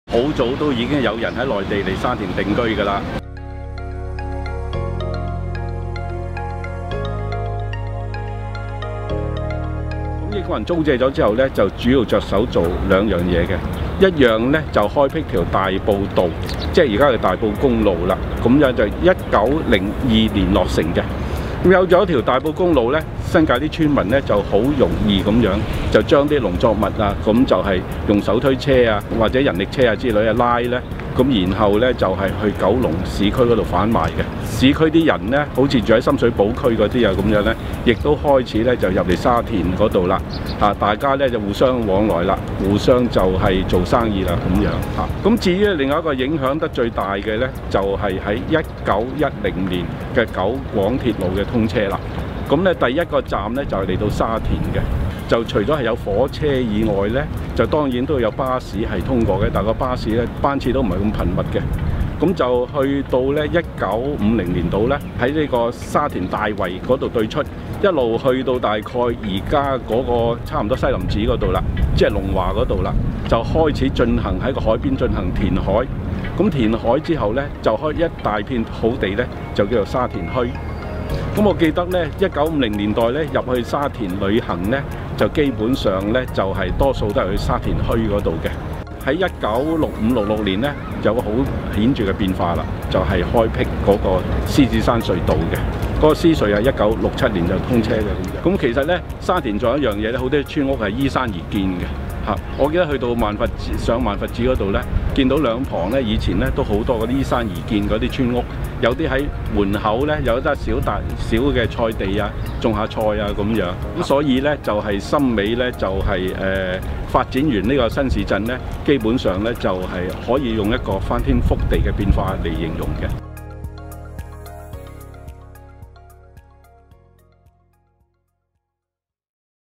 【鄭寶鴻X沙田變幻時】EP 1: 翻天覆地的沙田
今日嘅大埔公路、九廣鐵路、獅隧並唔係憑空而來！

EP 8: 銅鑼灣「東角」隻角喺邊？